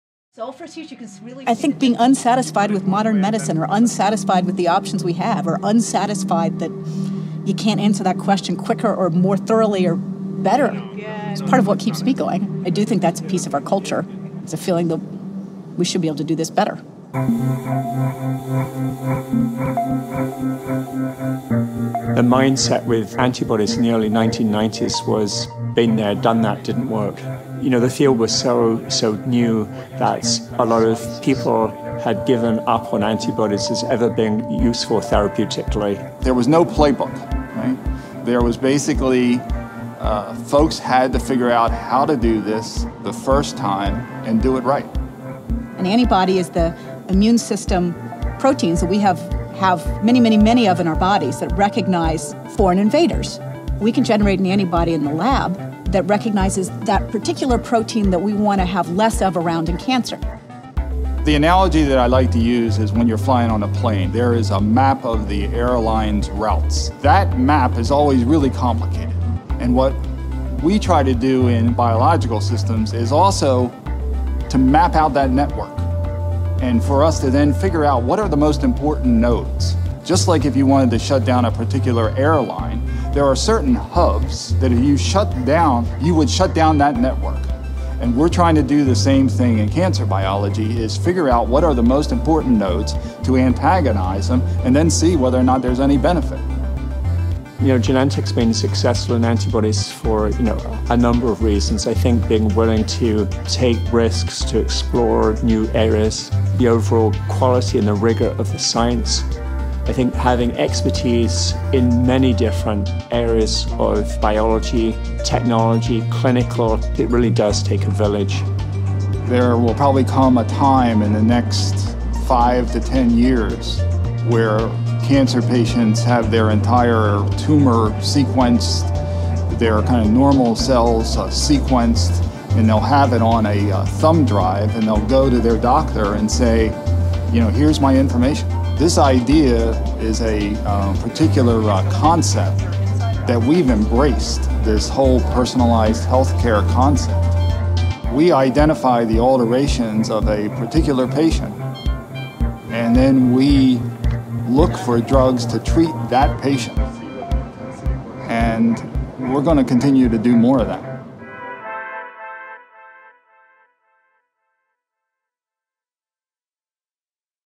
Designing Biologics: Large Molecule Drug Discovery
- Large molecule -- or biologic -- medicines include antibodies, fusion proteins and antibody-drug conjugates. We pioneered the use of genetic engineering to make medicine, and continue to create innovative biologics to treat disease.